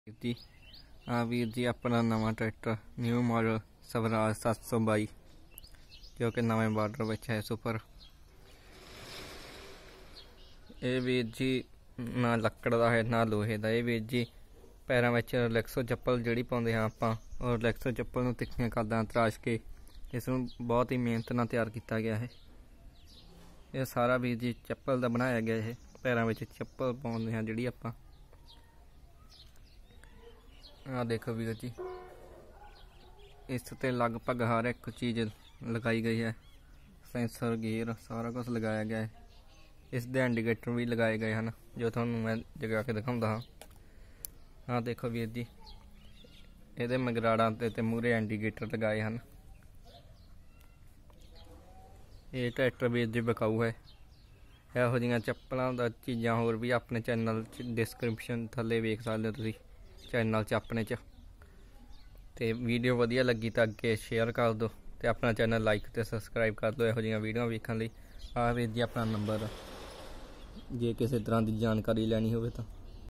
New model sawraj 722 super relxo ਚੱਪਲਾਂ ਦਾ
ਇਹ ਟਰੈਕਟਰ ਵੀਰ ਜੀ ਘਰੇ ਪੈਰਾਂ ਵਿੱਚ ਪਾਉਣ ਵਾਲ਼ੀਆਂ ਚੱਪਲਾਂ ਦਾ ਤਿਆਰ ਕੀਤਾ ਗਿਆ ਹੈ ਇਹ ਵਿਕਾਉ ਹੈ ਇਹ ਨੰ-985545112 ਤੇ ਹੋਰ ਜਾਣਕਾਰੀ ਲੈ
 ਸਕਦੇ ਹੋ